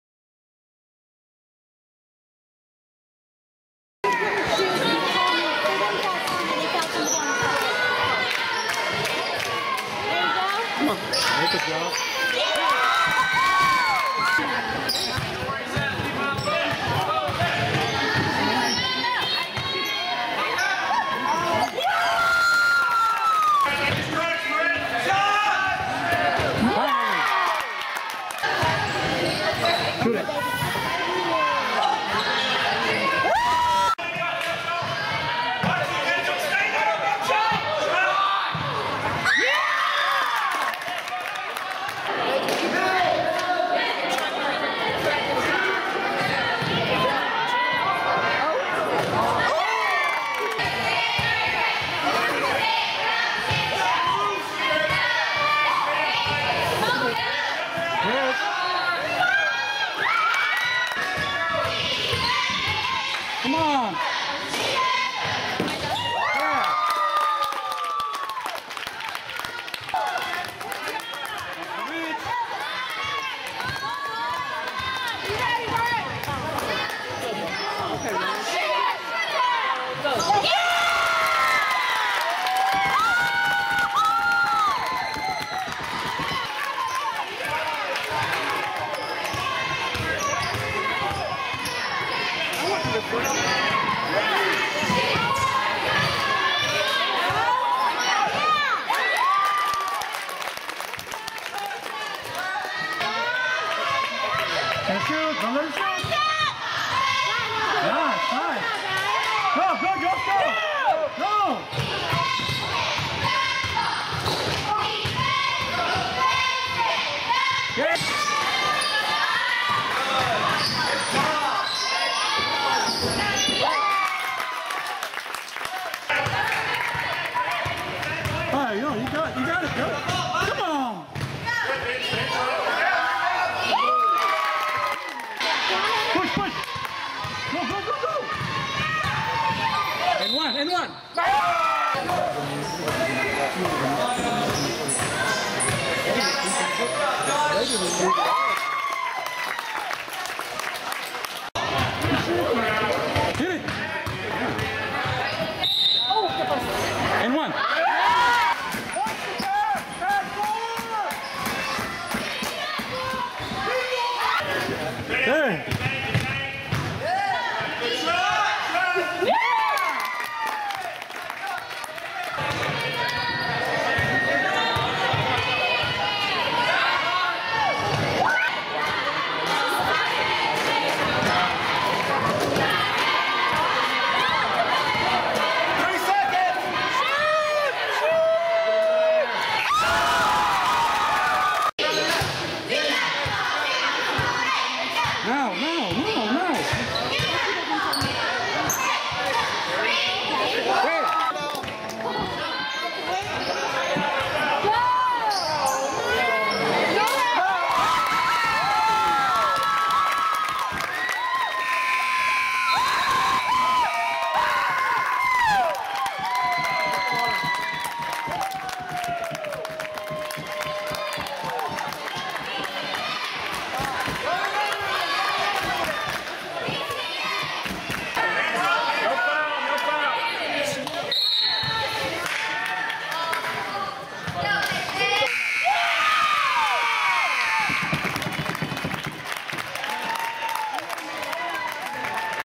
Class of 2015 Point Guard Josh Cortez Varsity Basketball Highlights
Junior Josh Cortez basketball highlights, Josh is a varisty three year starter and co-captain.  He will begin his senoir year August 2014.  He has a 4.0 GPA and is enrolled in dual credit courses, while taking honor classes.